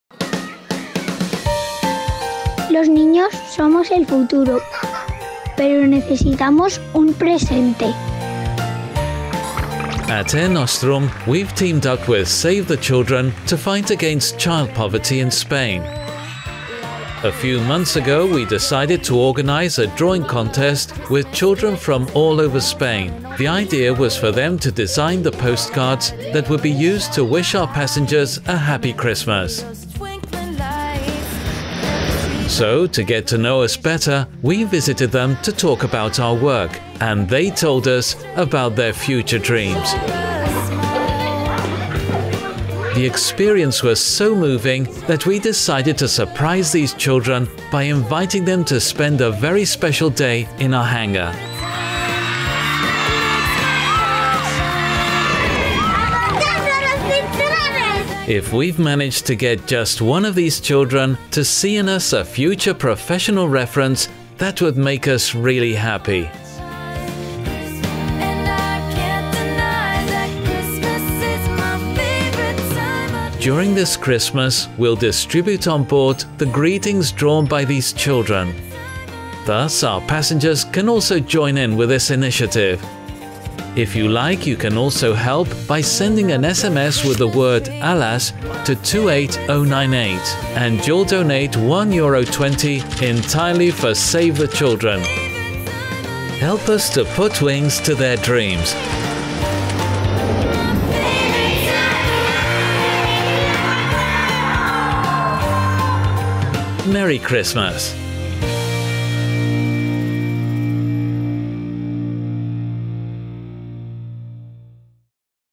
Help us to put wings to their dreams - Air Nostrum Christmas 2018
Children are the future, but they need to be the present.

At Air Nostrum we’ve teamed up with Save the Children to fight against child poverty. A few months ago, we decided to organise a drawing contest with children from all over Spain.

The idea was for them to design the postcards that would be used to wish our passengers a Happy Christmas.

So, to get to know us better we visited them to talk about our work… and they told us about their future dreams

The experience was so moving that we decided to surprise these children by inviting them to spend a very special day in our hangar.

If we’ve managed to get just one of these children to see in us a future professional reference… that would really make us happy.

During this Christmas we’ll distribute on board the greetings drawn by these children. Thus, our passengers can also join in with this initiative.

Help us to put wings to their dreams.

Merry Christmas!